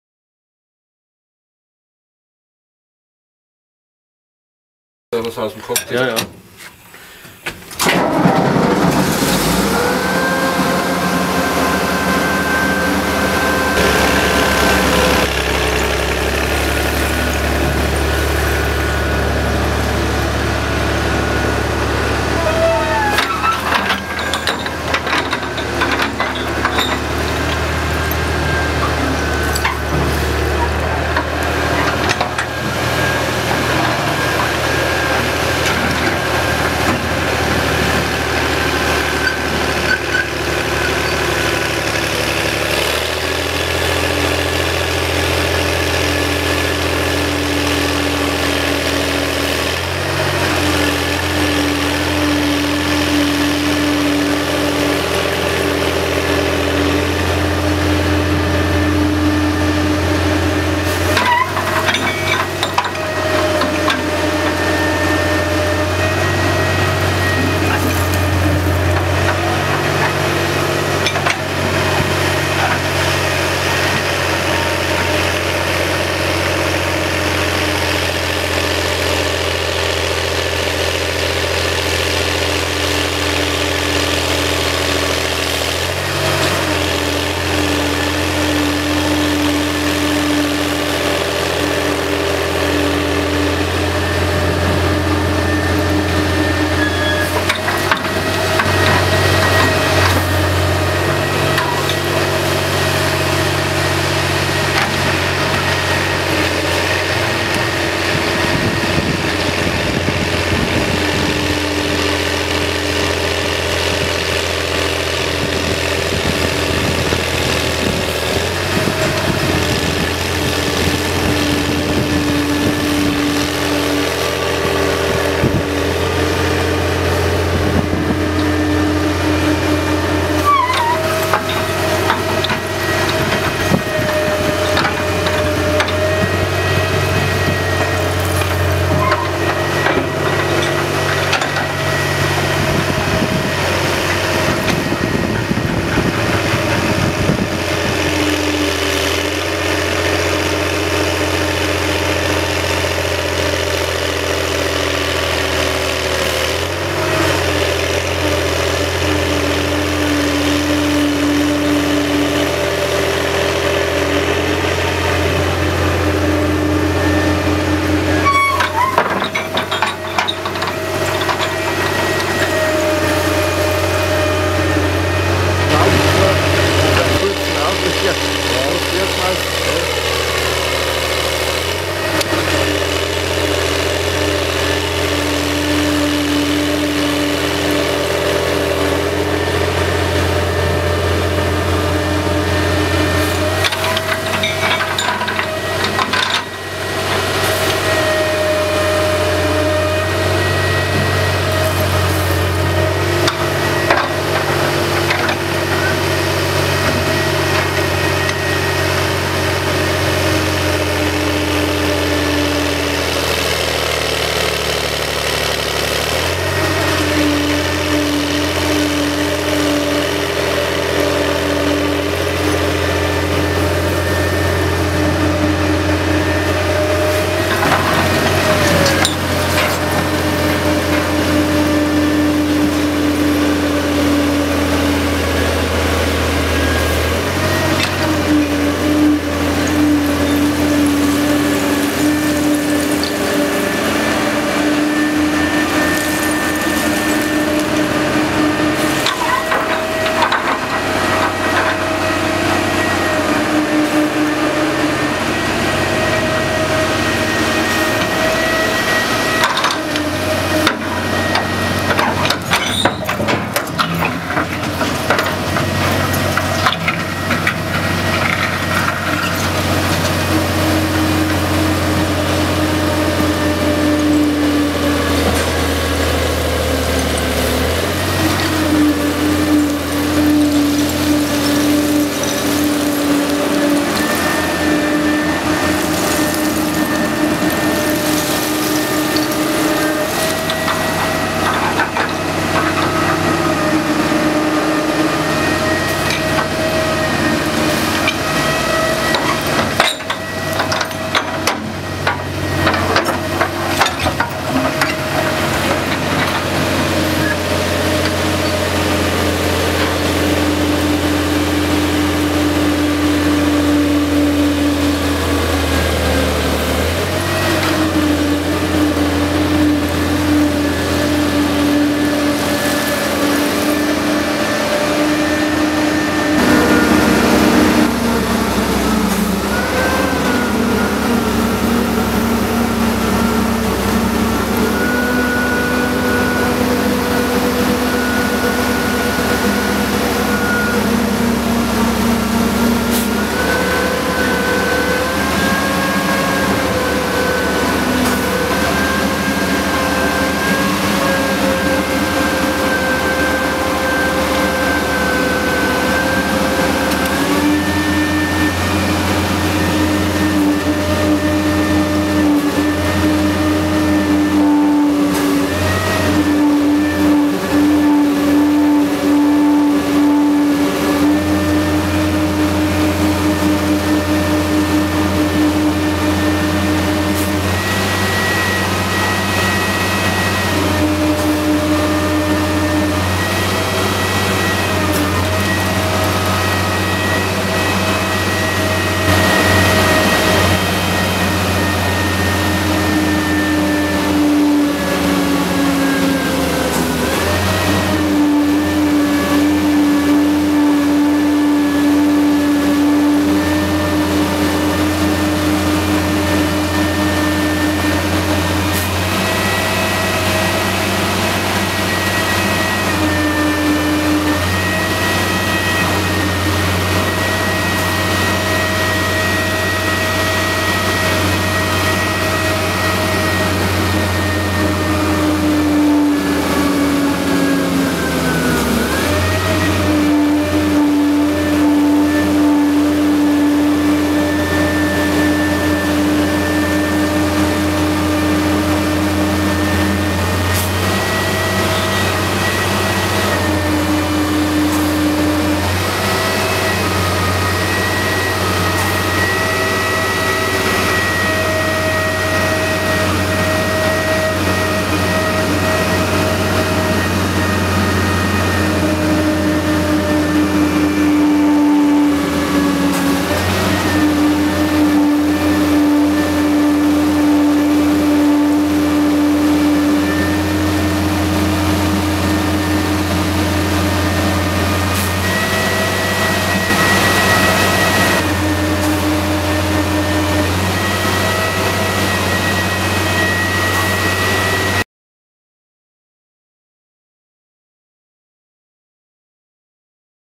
Menck M260 Seilbagger mit Schürfkübel Vintage Menck Dragline at work
Menck M260 Seilbagger mit Schürfkübel der Fa. Luff